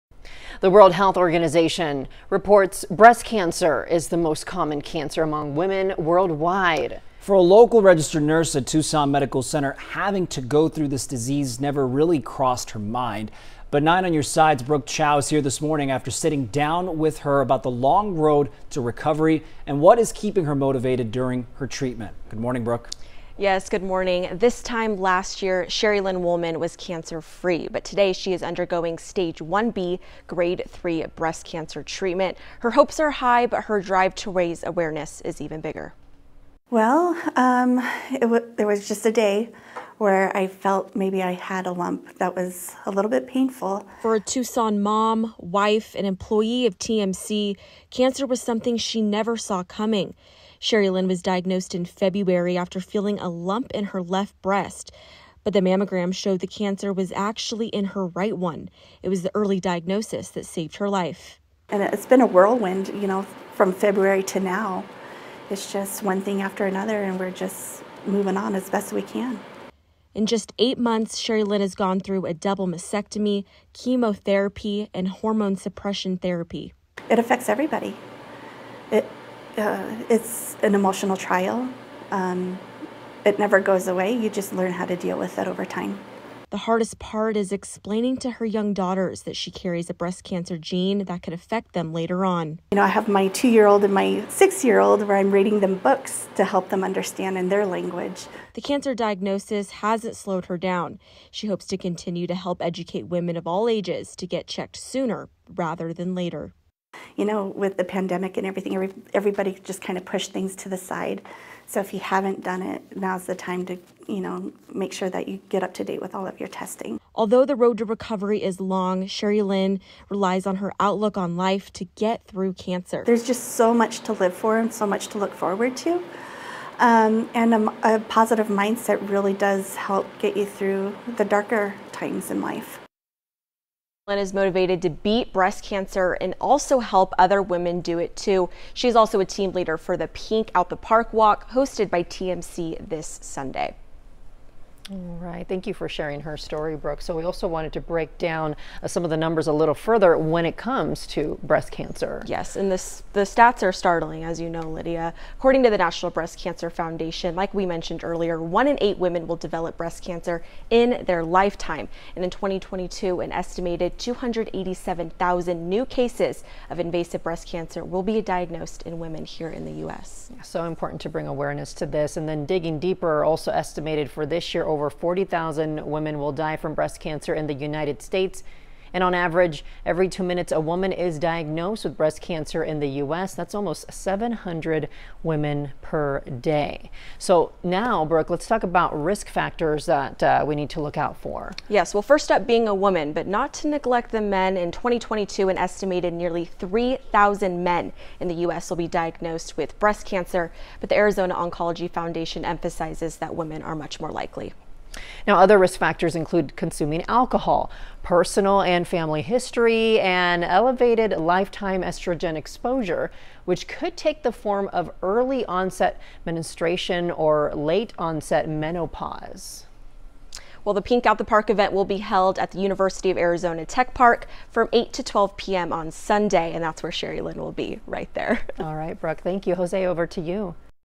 Local woman's breast cancer journey through self diagnosis
The American Cancer Society's estimates that 1 in 8 women in the United States will develop breast cancer in their lifetime.

“There’s just so much to live for and so much to look forward to and a positive mindset really does help you get through the darker times in life," said Wollman.

Consider helping KGUN 9 'Pink Out the Park' on Sunday, October 16.